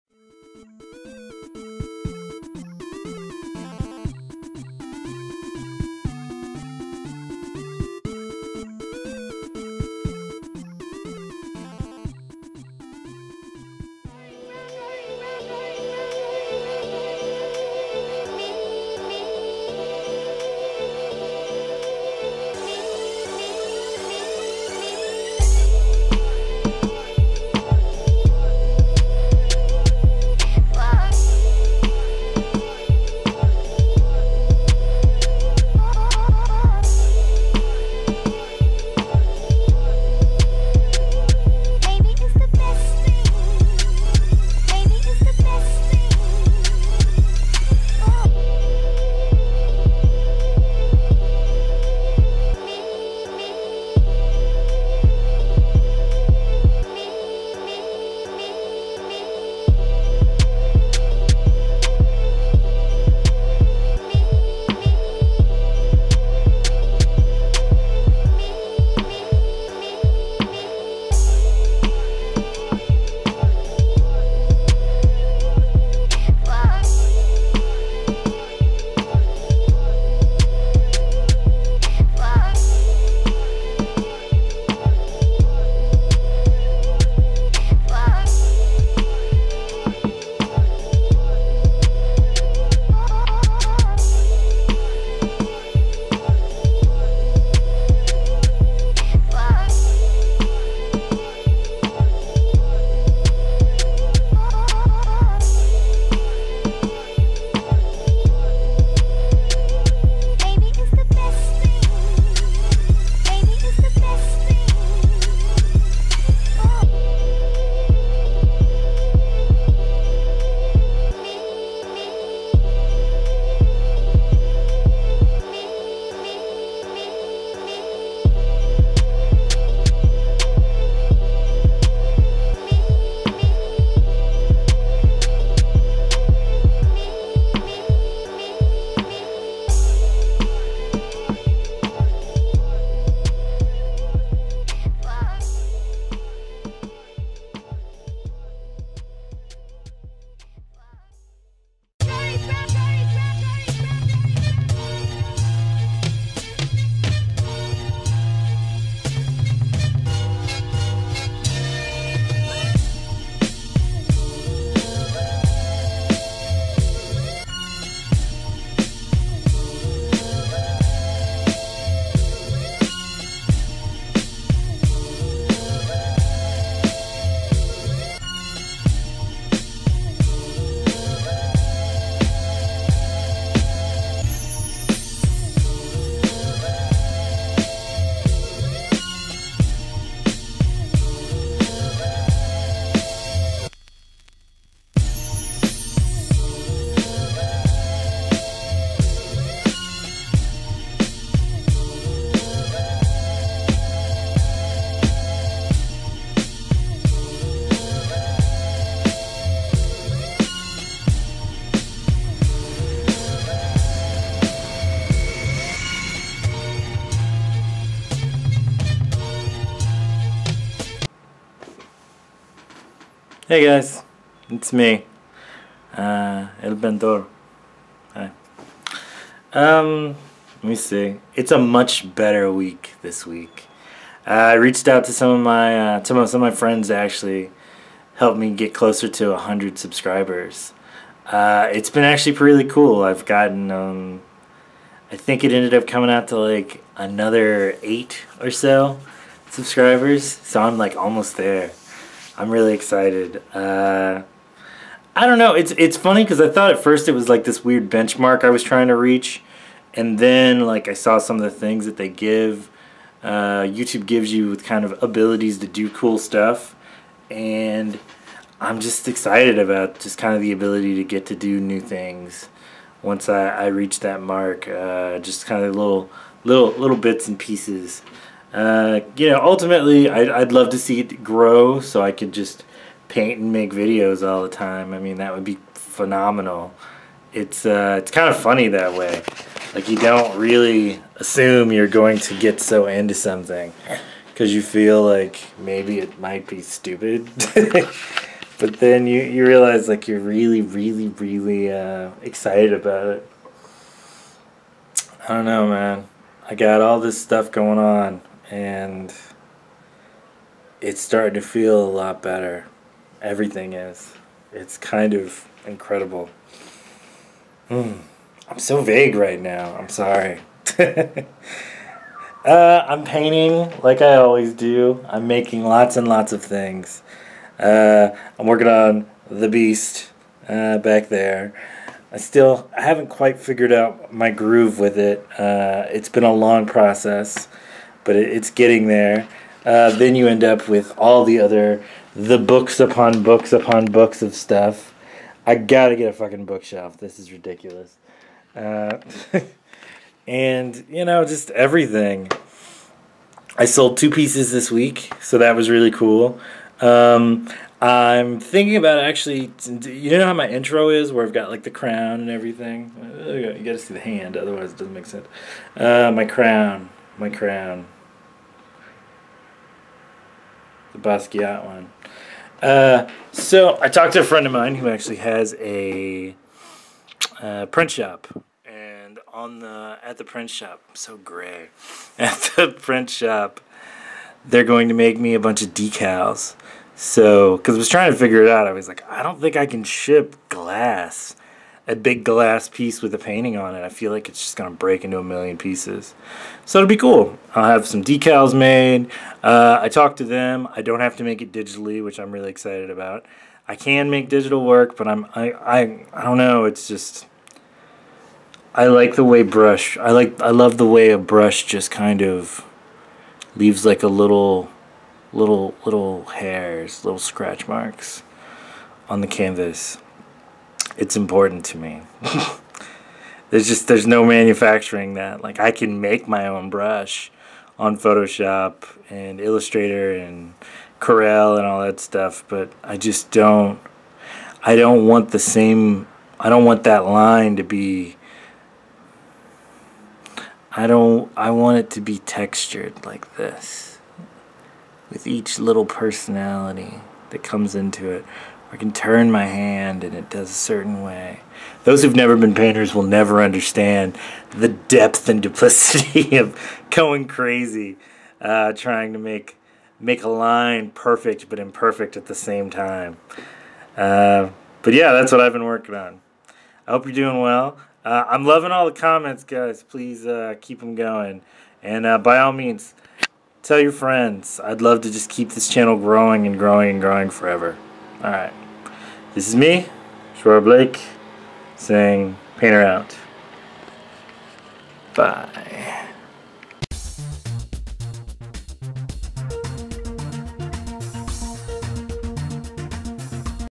Jaws Blake- inkwash, watercolor paintings, illustration
I've been working hard this week making lots of different work. I try to keep it moving all the time so that my mind is on fire with shapes and colors.

New people feel free to chat in the comments. Long time people I would love to keep hearing from you. Love you all. See you around.